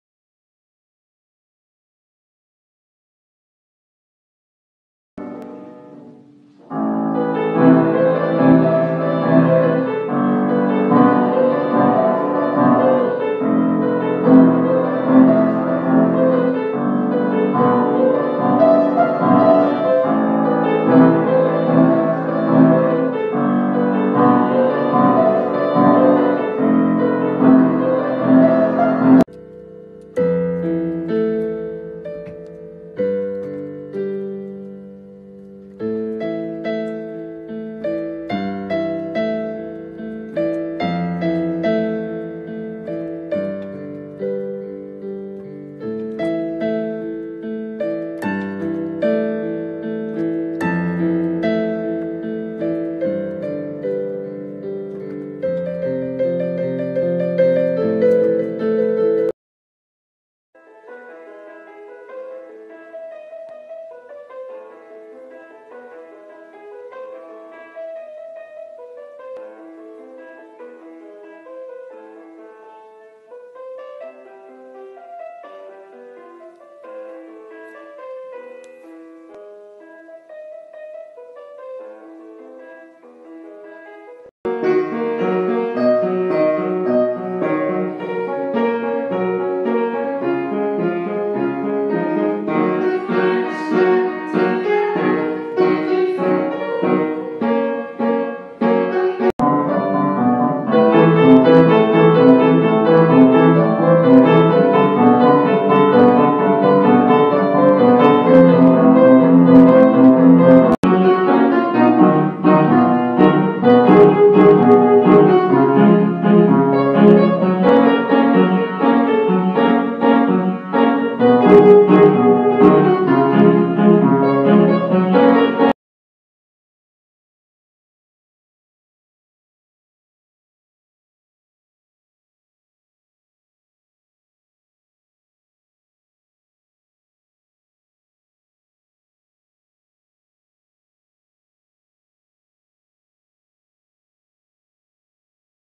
Le piano et moi piano facile
Moi au piano apprendre le piano n'est pas facile
 Apprendre à jouer du piano, c’est expérimenter le goût de l’effort, la discipline, la ténacité et l’humilité.
Le piano, c’est aussi s’exposer aux autres, à travers un sens qui suscite l’émotion. Jouer, c’est être écouté, se confronter à la peur du jugement, à la crainte de ne pas être à la hauteur, c’est accepter les fausses notes et renoncer à la perfection.
c’est ainsi un fabuleux moyen de travailler sur soi.